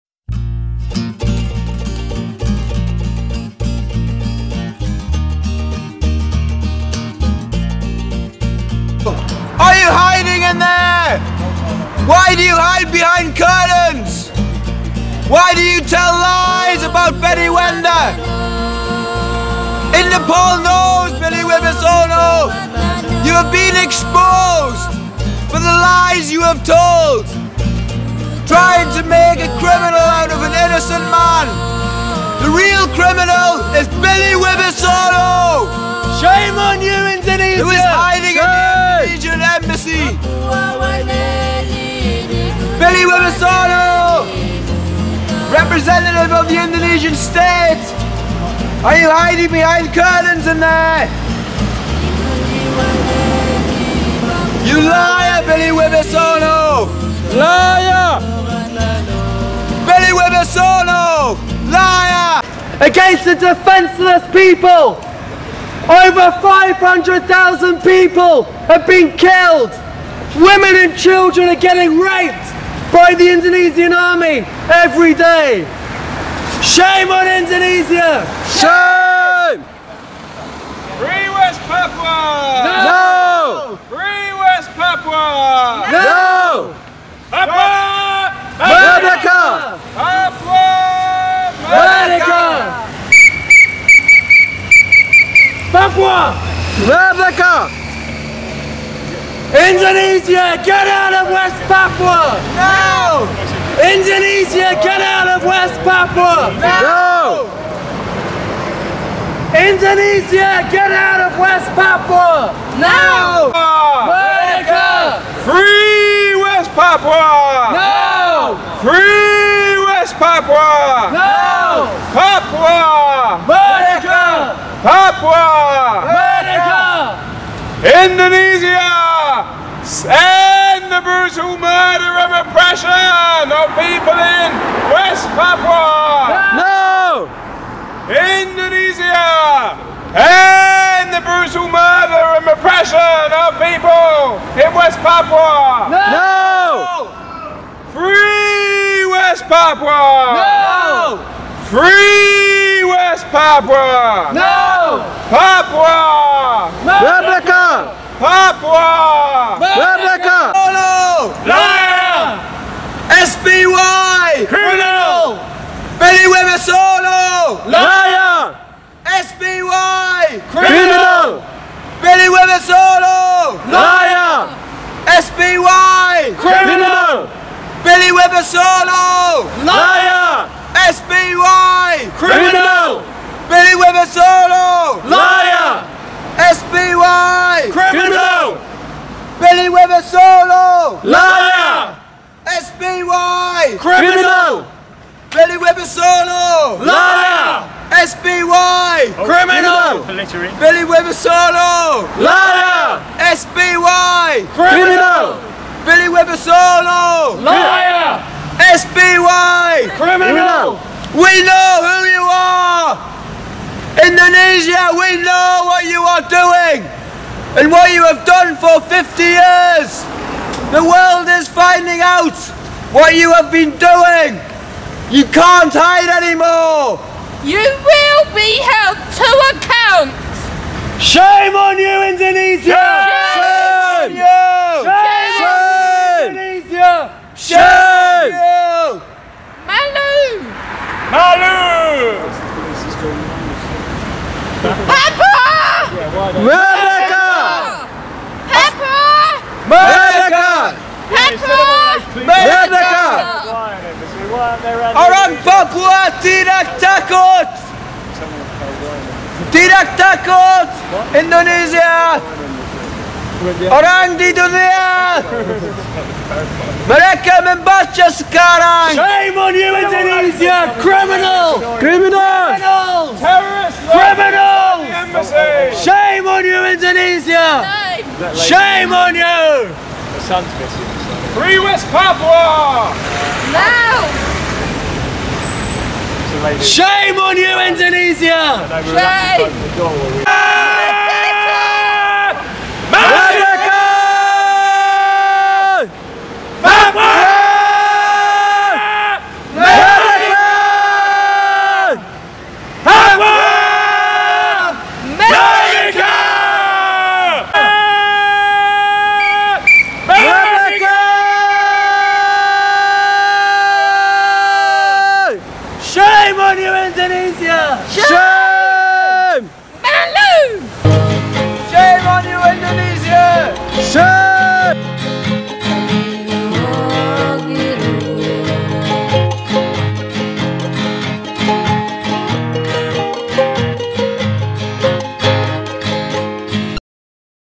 INDONESIA CRIMINAL WAR CRIMES STATE.mpg
WestPapuaFreedom:

Indonesia Criminal War Crimes state, because Indonesia managed to kill over 500.000 innocent civilian, also Indonesia Military and Police raping young woman and children every day in West Papua. Indonesian Embassy running by CRIMINAL war crimes.